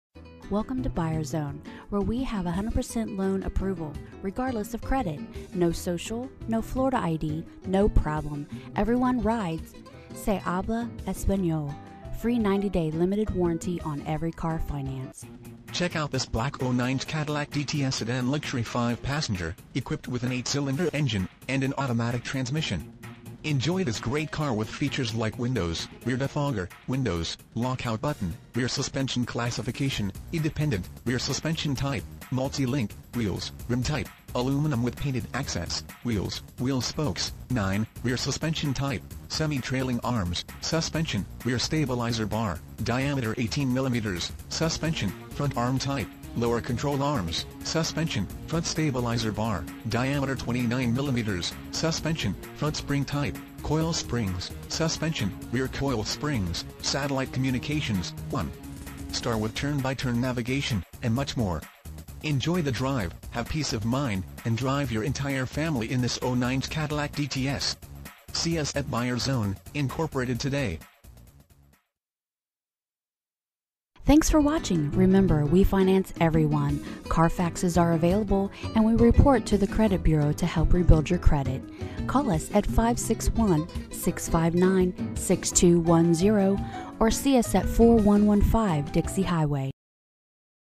2009 Cadillac DTS Buyers Zone, Inc. West Palm Beach, FL 33405
Cadillac DTS Check out this Black Cadillac Dts Sedan, equipped with an 8 Cylinder engine and an automatic transmission. Enjoy this great car with features like Windows, Rear Defogger, Windows, Lockout Button, Rear Suspension Classification: Independent, Rear Suspension Type: Multi-Link, Wheels, Rim Type: Aluminum With Painted Accents, Wheels, Wheel Spokes: 9, Rear Suspension Type: Semi-Trailing Arms, Suspension, Rear Stabilizer Bar: Diameter 18 MM, Suspension, Front Arm Type: Lower Control Arms, Suspension, Front Stabilizer Bar: Diameter 29 MM, Suspension, Front Spring Type: Coil Springs, Suspension, Rear Coil Springs, Satellite Communications, OnStar W/Turn-By-Turn Navigation, Suspension, Self Leveling Suspension, Tires, All Season, Power Windows, Windows, Front Wipers: Intermittent, Windows, Laminated Glass, Tires, Tire Pressure Monitoring Systeming System, Exterior Mirrors, Heated, and much more. Enjoy the drive, have peace of mind, and drive your entire family in this Cadillac Dts. See us at Buyers Zone, Inc. today!VIN:1G6KD57Y99U129112

Buyers Zone, Inc.
4115 South Dixie Highway
West Palm Beach, Florida 33405